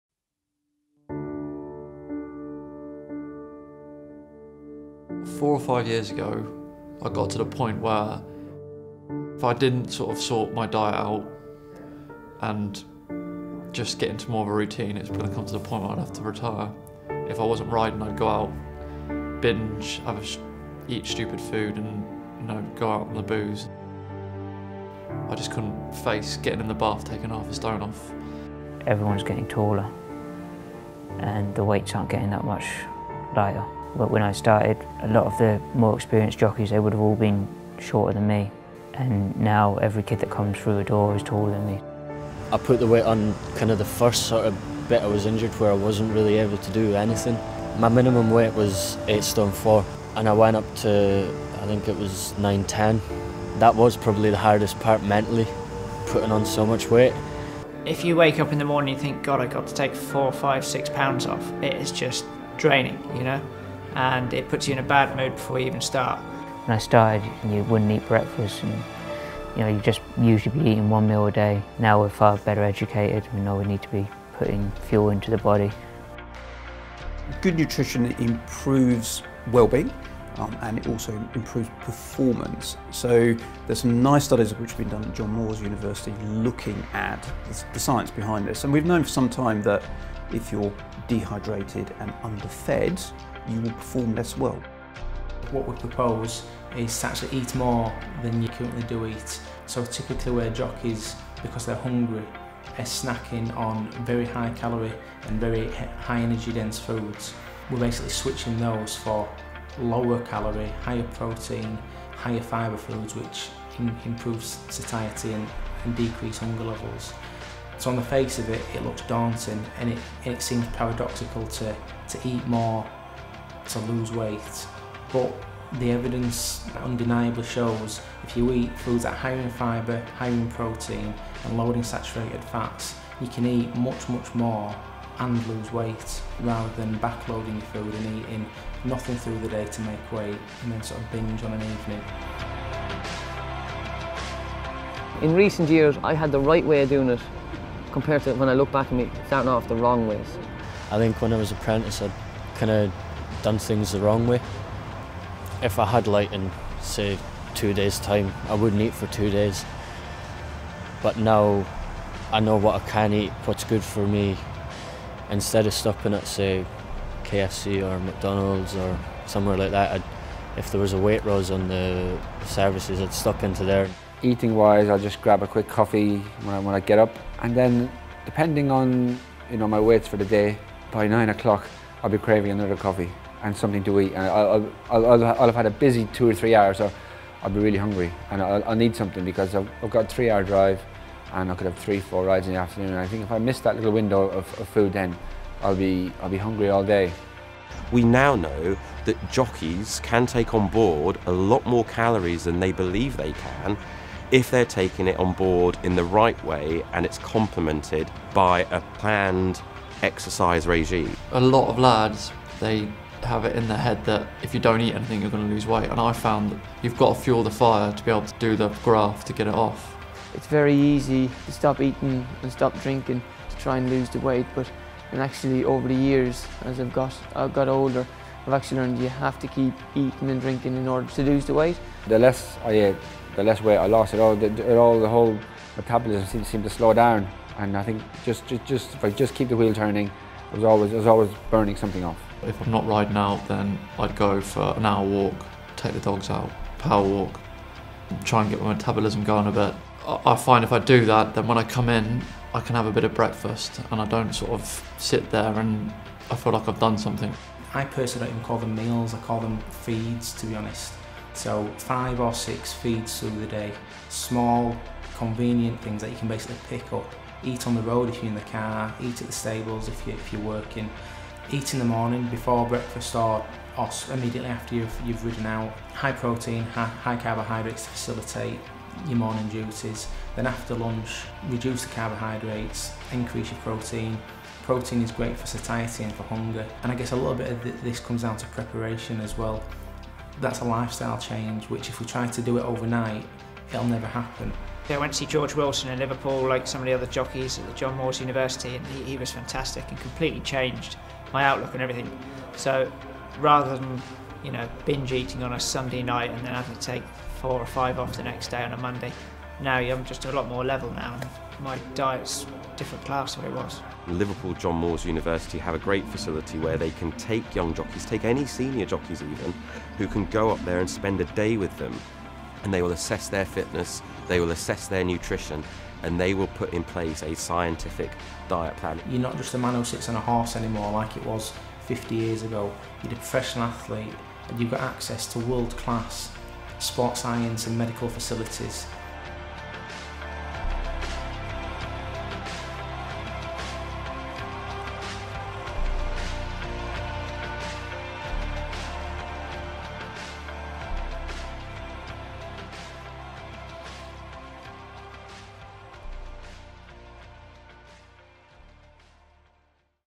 JOCKEY MATTERS: NUTRITION - Making Weight the Right Way
Part of the Jockey Matters series.  Hear from a range of jockeys and nutrition experts on how jockeys can better manage their calorie intake and how diet can impact on performance and long term physical and mental wellbeing.  Also find out where jockeys can turn to for support.